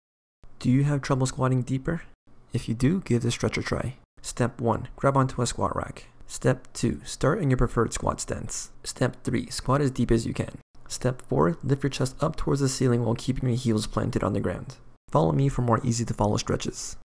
How to SQUAT Deeper!
Do you have difficulty squatting deeper?

The Pole Squat is one of the most effective ways to help deepen your squat as it helps mobilize the ankles, hips and thoracic spine.

✅ 𝗥𝗘𝗛𝗔𝗕 & 𝗠𝗢𝗕𝗜𝗟𝗜𝗧𝗬 𝗣𝗥𝗢𝗚𝗥𝗔𝗠𝗦 - 𝗦𝗧𝗔𝗥𝗧 𝗠𝗢𝗩𝗜𝗡𝗚 𝗣𝗔𝗜𝗡 𝗙𝗥𝗘𝗘 𝗡𝗢𝗪!!